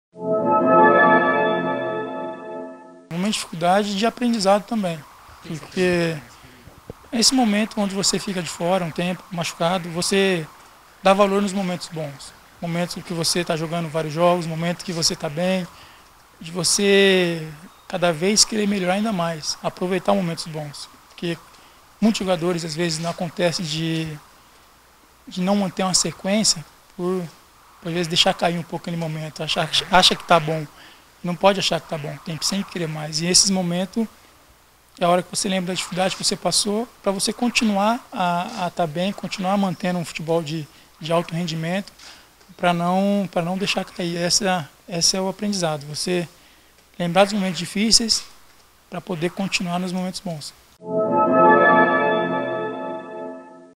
Gum: Nunca pode achar que já está bom
Jogador fala sobre o aprendizado no Fluminense